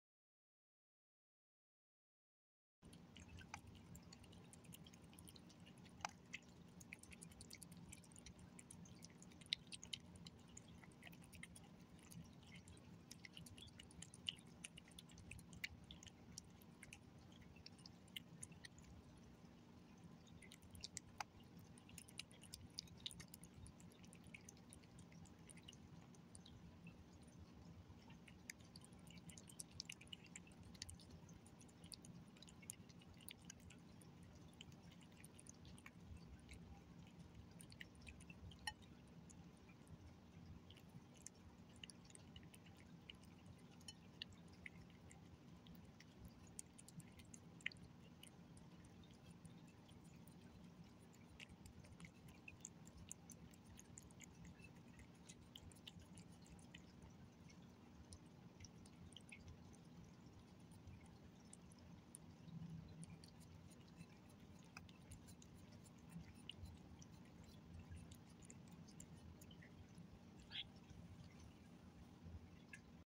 Tabby Cat Eats Tuna Florentine
Frisky eats tuna florentine with spinach.

Frisky is a sixteen month old gray tabby male cat.  12/8/2022